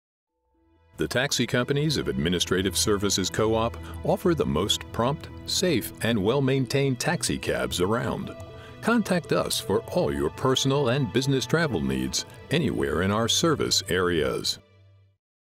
Administrative Services Co-Op -- California
Our co-operative network of taxi service providers is here to get you where you need to be 24/7/365. With service available across southwestern California, if you are looking for a reliable ride at a fair price, you should call us first!